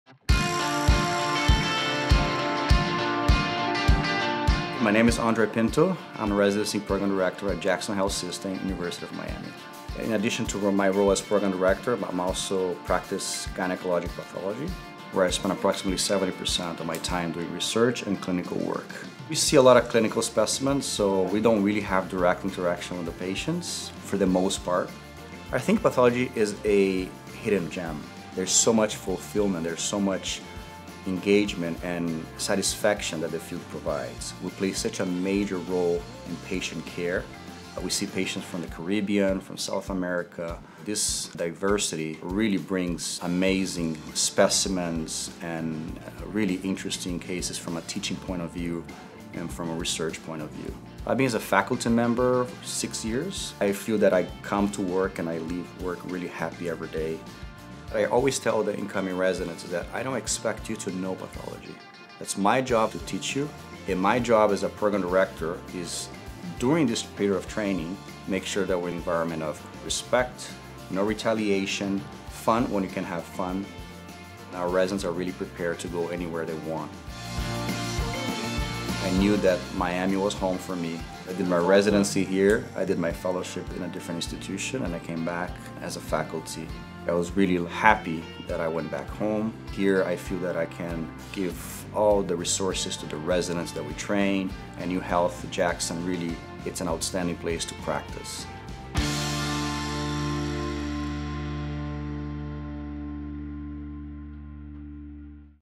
Why Study Pathology in Miami? Featuring Dr. Andre Pinto
Director of Pathology Residency Training Program for The Department of Pathology and Laboratory Medicine.